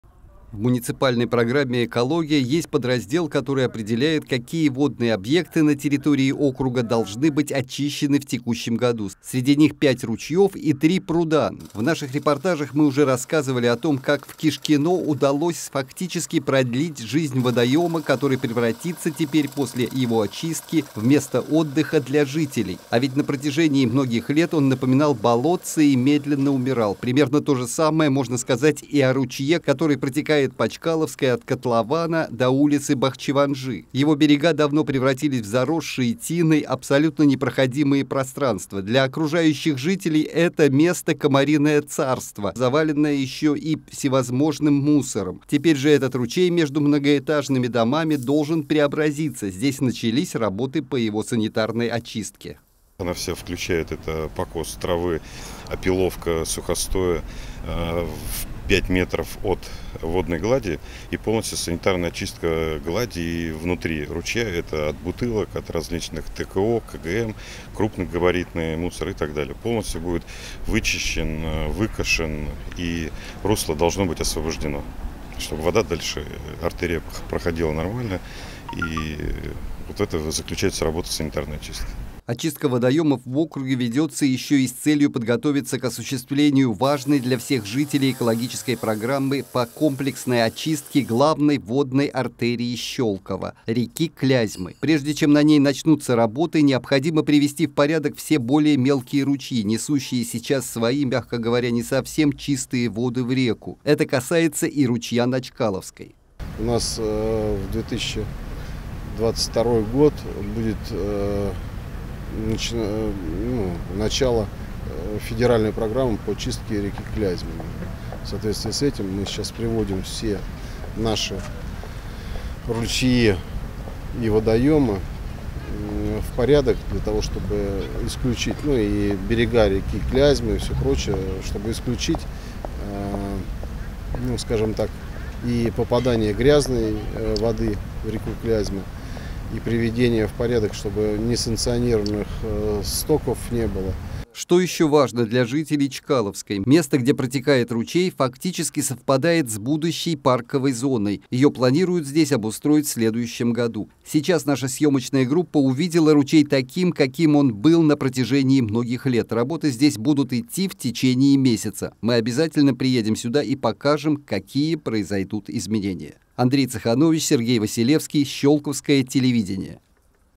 По программе «Экология»
В микрорайоне Чкаловский началась очистка ручья, который протекает от Чкаловского озера до улицы Бахчиванджи.